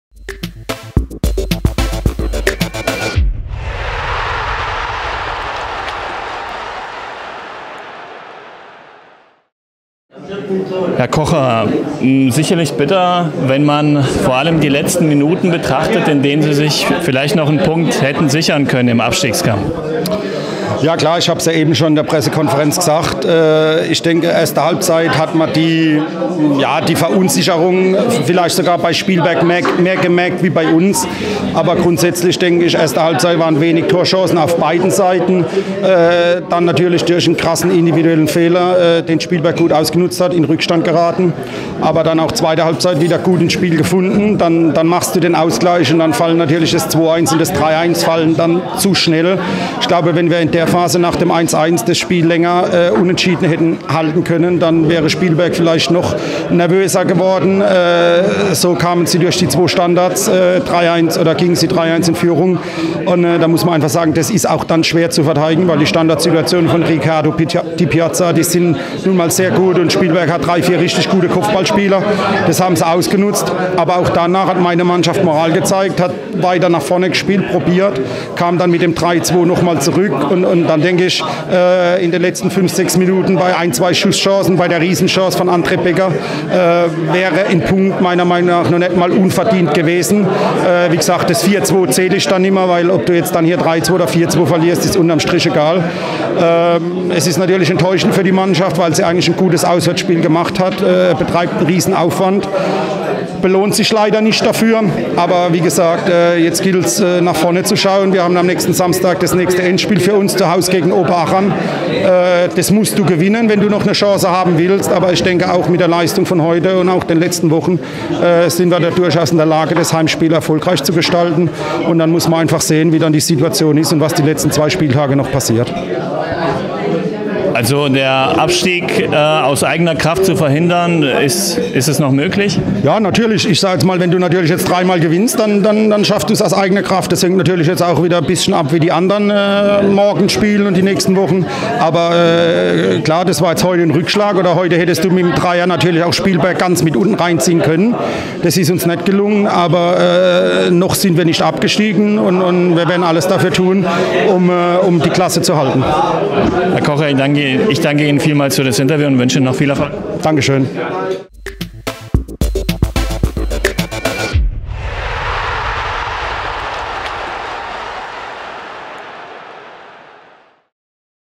Andreas Kocher - FC-Astoria Walldorf II - zum Spiel vs. SV Spielberg, FuPa.tv-Interview, 28.4.2017
"Ein Punkt wäre nicht unverdient gewesen."

Andreas Kocher, Trainer des FC-Astoria Walldorf II, im FuPa.tv-Interview nach der Begegnung SV Spielberg - FC-Astoria Walldorf II am 31. Spieltag der Oberliga Baden-Württemberg 2016/17.